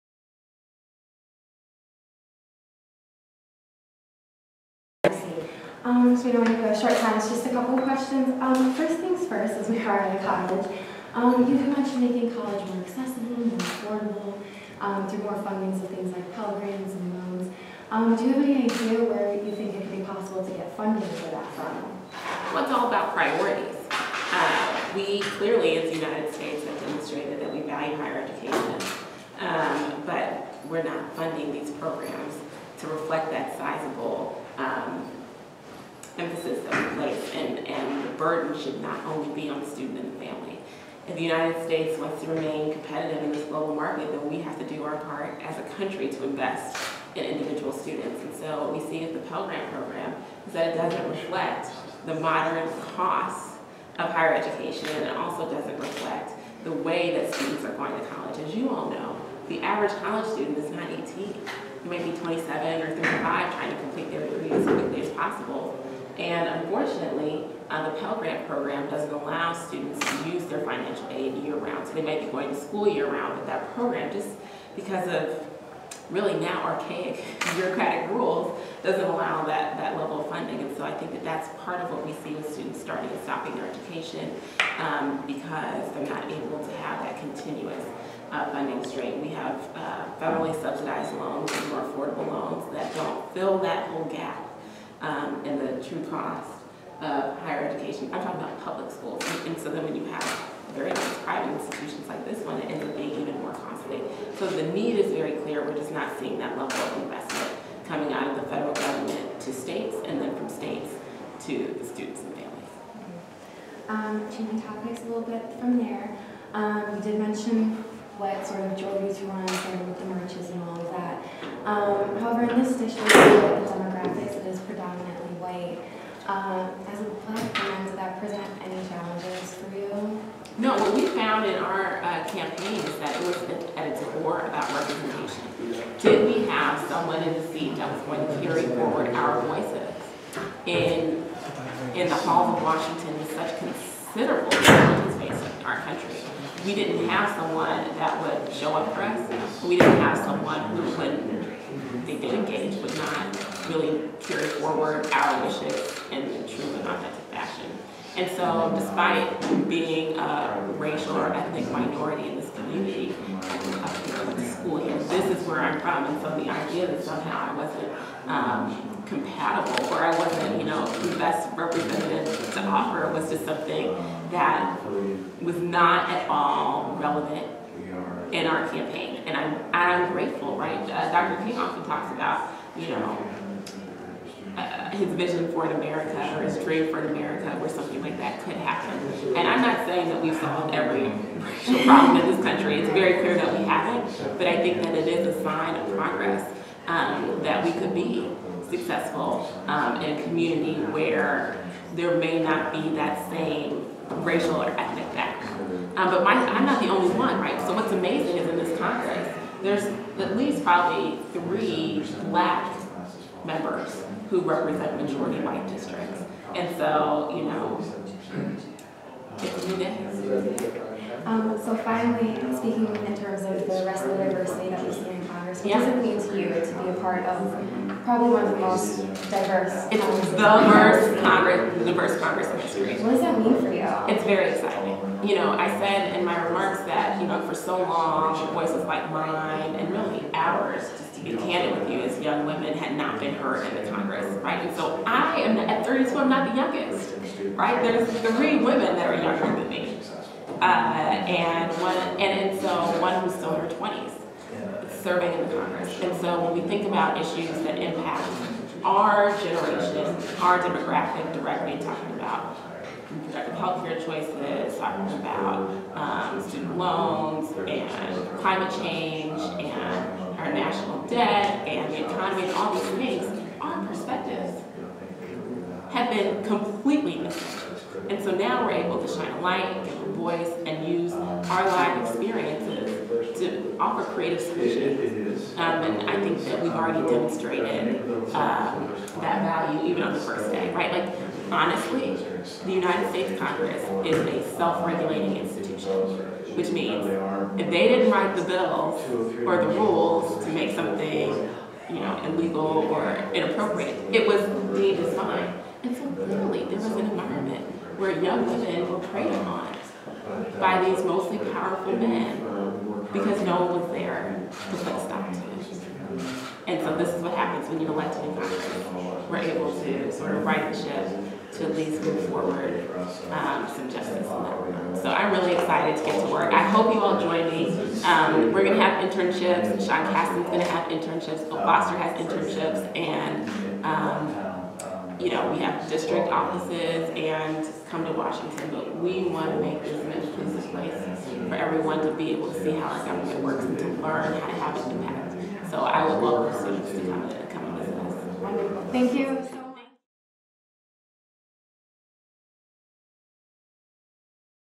Chronicle interview with Rep. Lauren Underwood
Chronicle/ncclinked reporters got an opportunity to speak one-on-one with Illinois Rep. Lauren Underwood at NCC's MLK Day Prayer Breakfast.